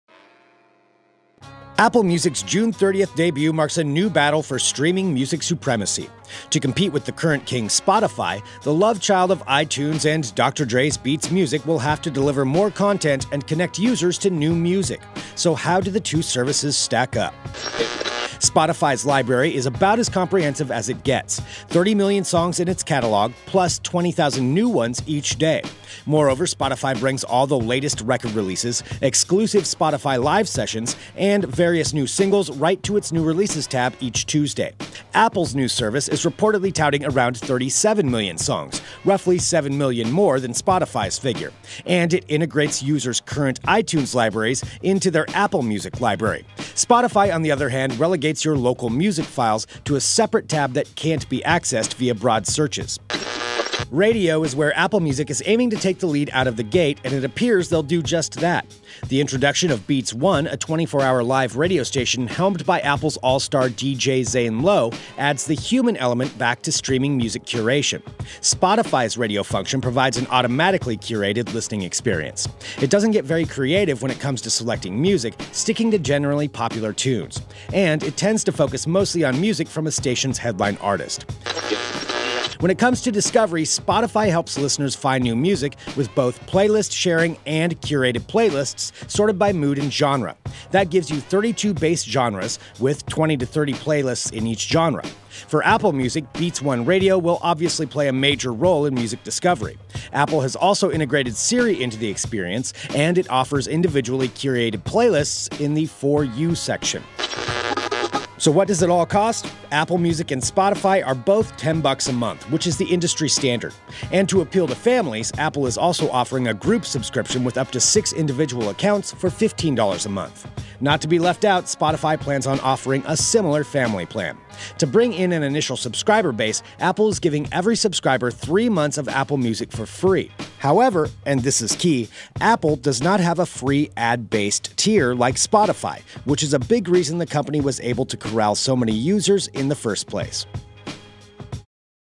Apple Music vs. Spotify - Streaming Music Showdown
For all intents and purposes, the undisputed king of streaming music these days is Spotify. With a reported paid user base surpassing an astounding 20 million subscribers — not to mention an astronomical 55 million additional listeners of its free, ad-based service — the Swedish-born service currently trounces its competition, most of which boast a few million users at best.

Now Apple has entered the fray, recently announcing its much anticipated new music service, Apple Music. However, while Apple Music has the benefit of the tech giant’s near-infinite resources behind it to go toe-to-toe with Spotify, whether or not it can upend the champ is the question of the hour.